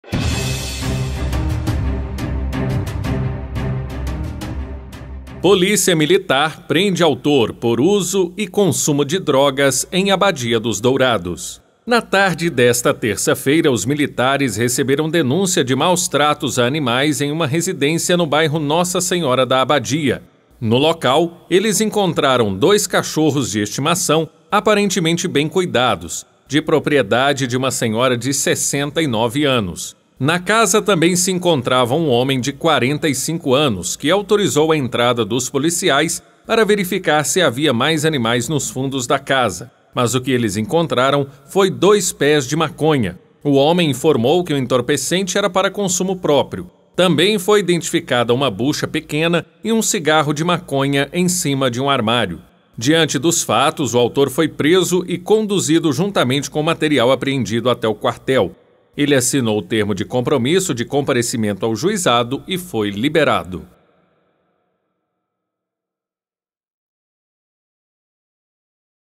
Polícia Militar prende homem por uso e consumo de drogas em Abadia dos Dourados
Caso aconteceu na tarde dessa terça-feira (25). Os militares foram acionados via denúncia anônima, por suposto maus tratos de animais, mas ao chegarem ao local, encontraram dois pés de maconha no quintal da residência.